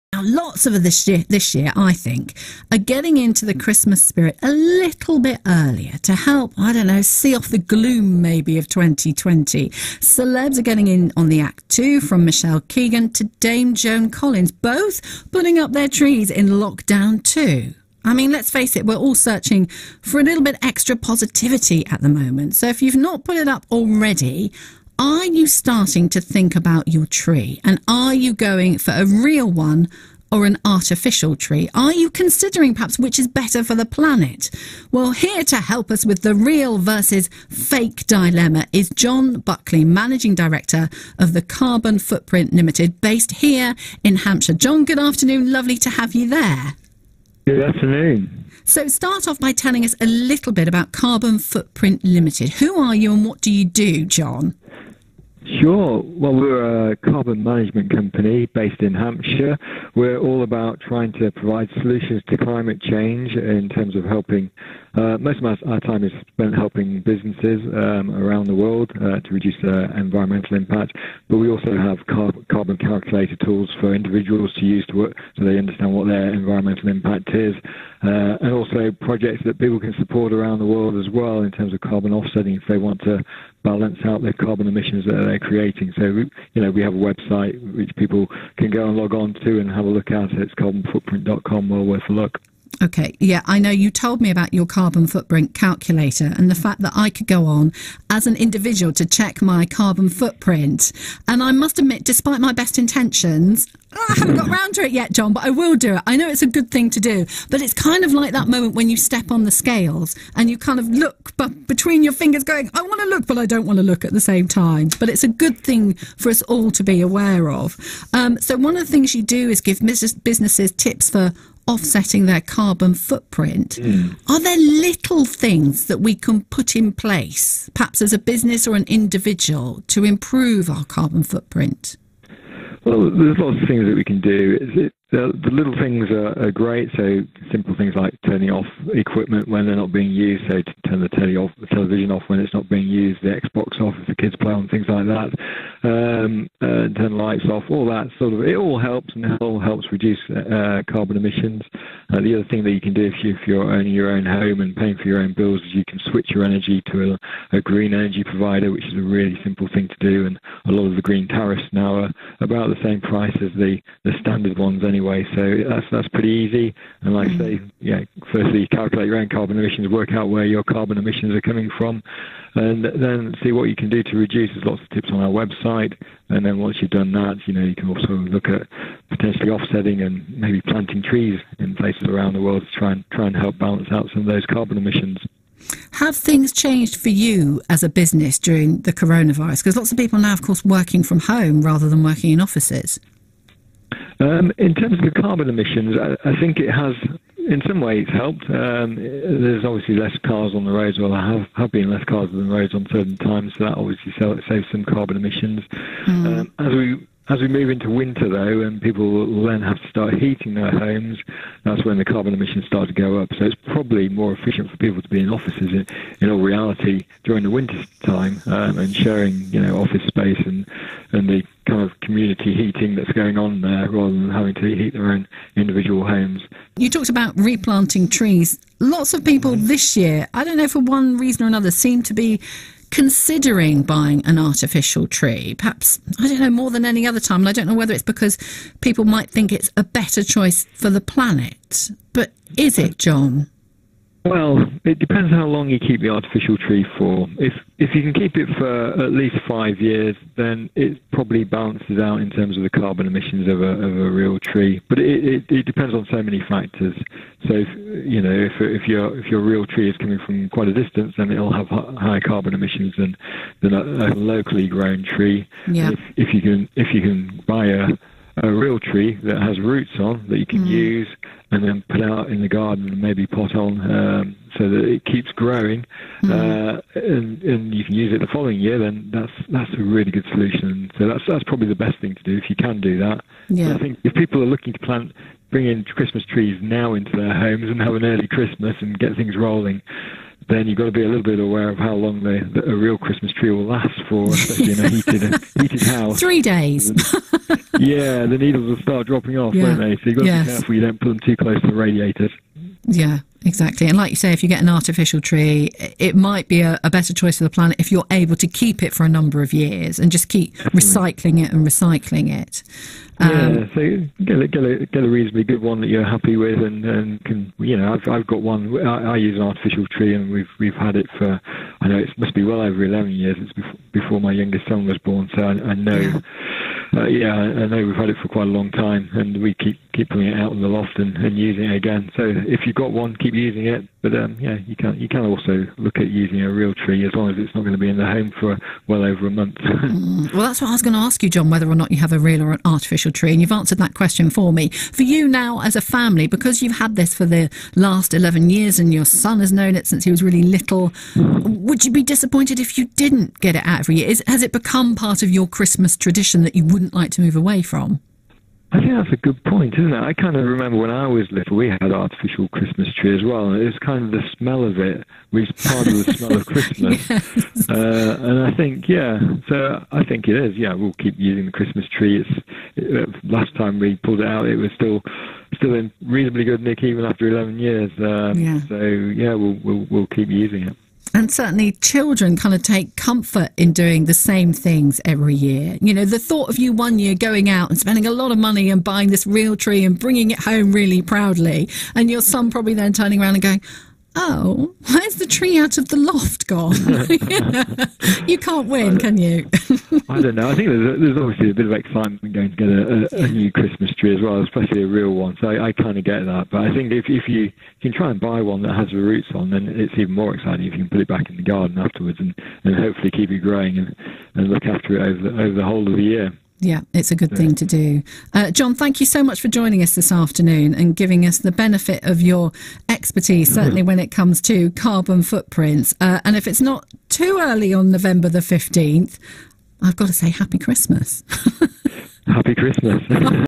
Carbon Footprint of Christmas Trees- BBC Radio Interview
BBC Radio Solent's Rebecca Parker interviews Carbon Footprint Ltd John Buckley and talks about measuring your carbon footprint, how to reduce it and the carbon footprint of Christmas trees.
Visit our 12 Carbons of Christmas webpage for top tips on reducing Christmas emissions and still having fun